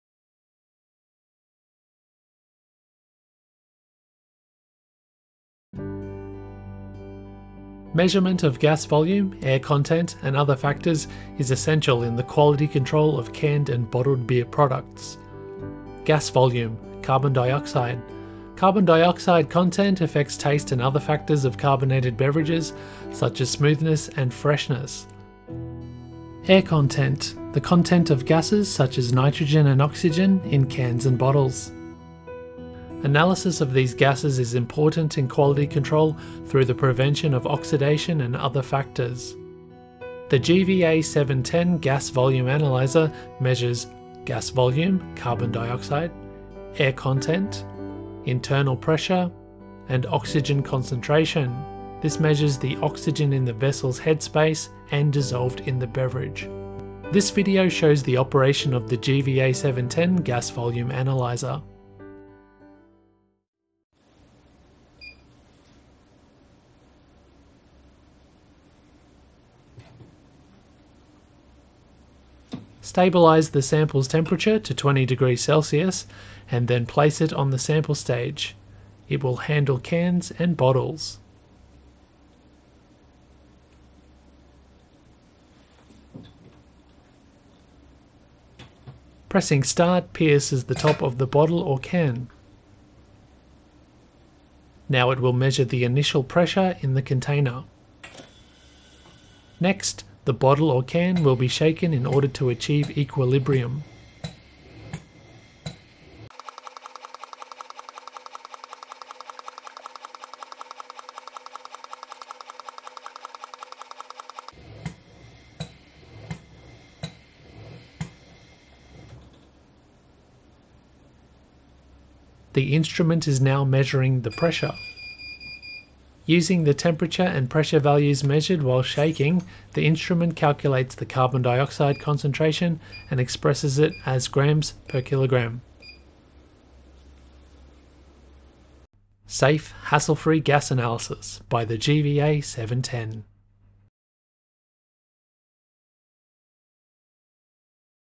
The measurement of gas volume in beer by the GVA-710 Gas Volume Analyzer
Measurement of gas volume, air content and other factors is essential in the quality control of canned and bottled beer products.
This video shows the operation of the GVA-710 Gas Volume Analyzer.